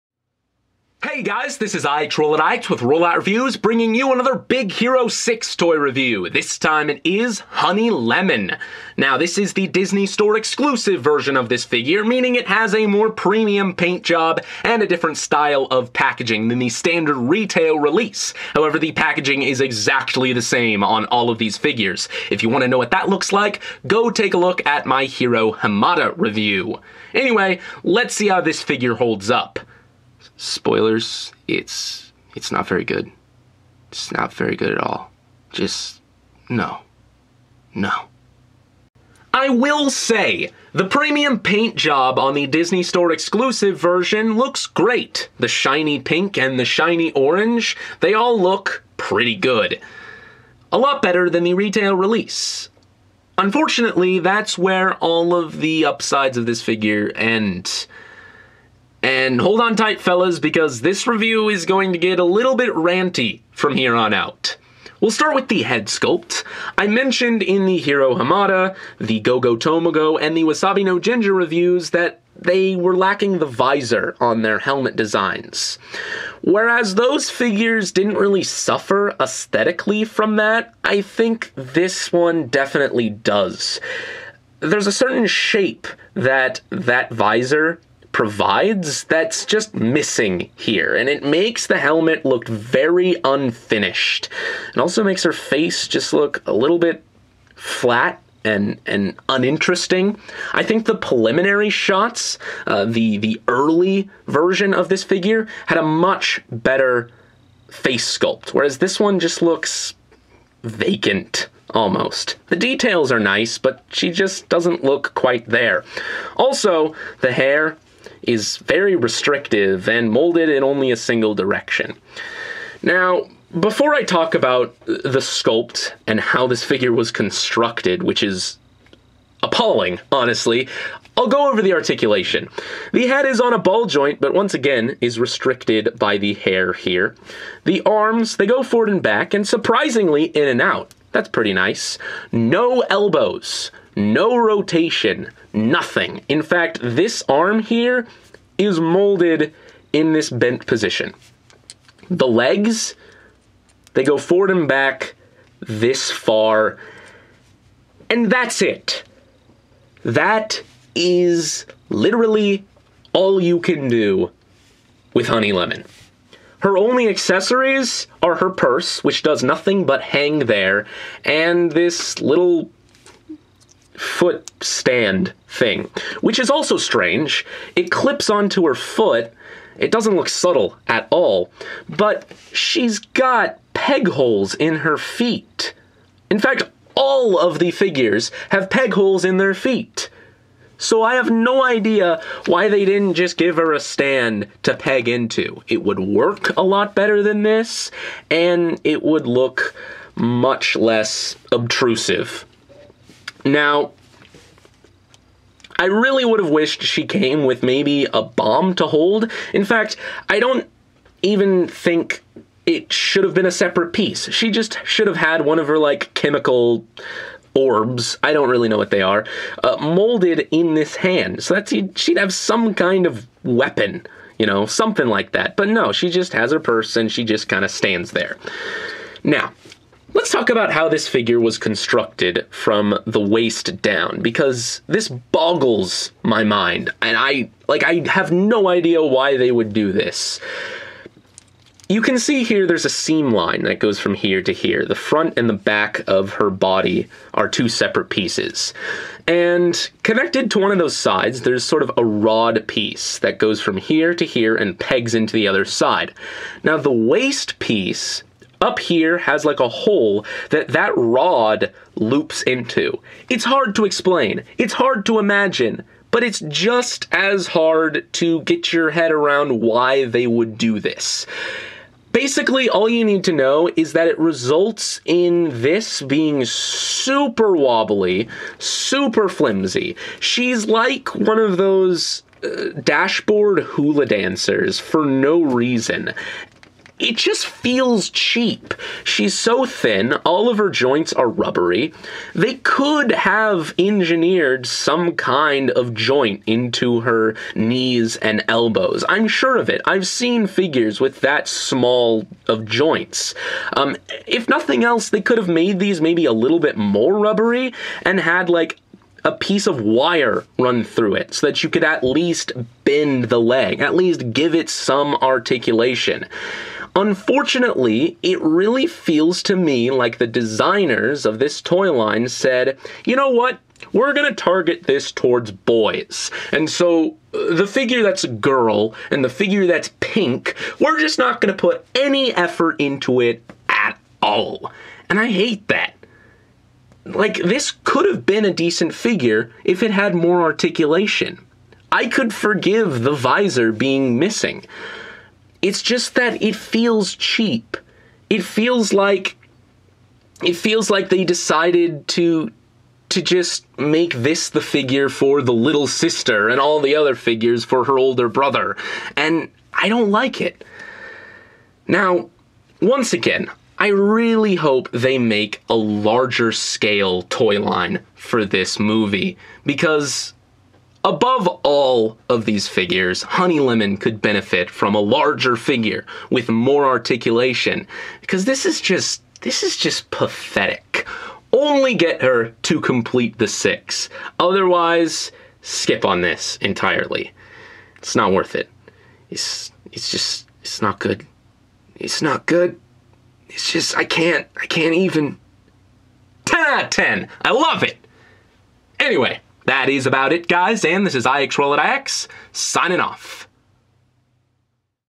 BIG HERO 6 Toy Review - Honey Lemon (Disney Store Ver.)
Maybe this kind of figure would appeal more to young girls?... I certainly wouldn't know.